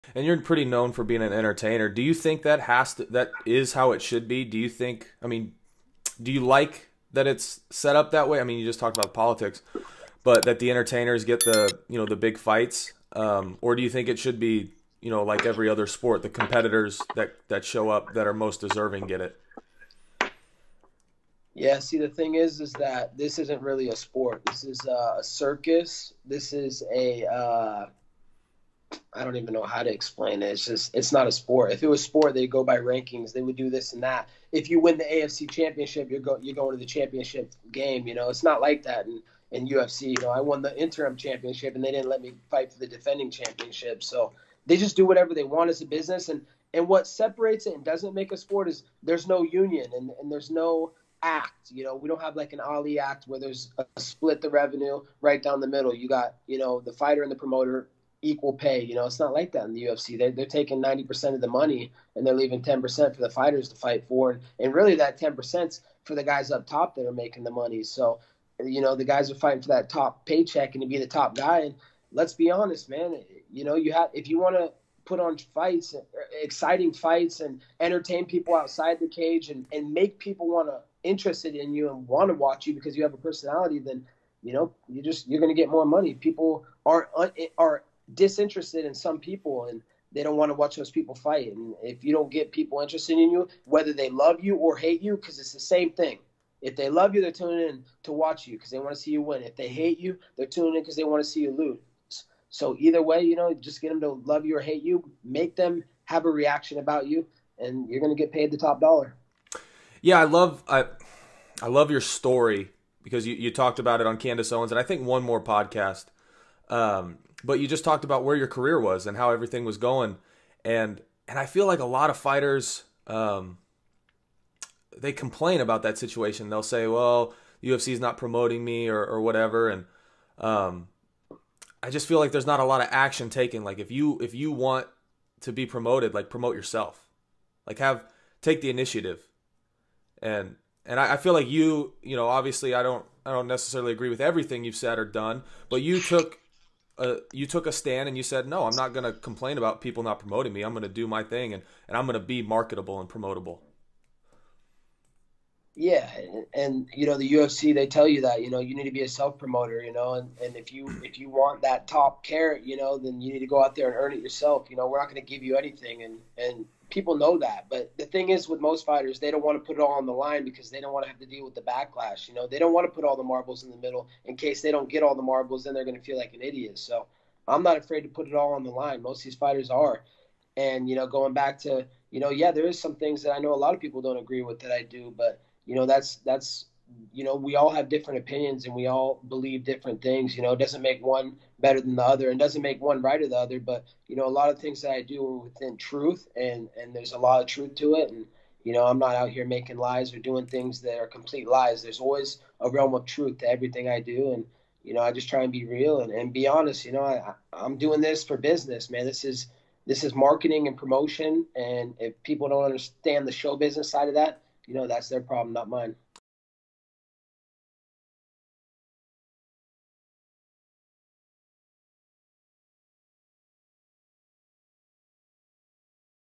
Did Colby Covington Take It Too Far At The UFC 296 Press Conference
As we are now done with UFC 296 I thought I'd share a clip from a podcast I did with Colby Covington A few years ago. Lmk if you think he went to far invoking Leon's late father in the presser.

The Unlikely Missionary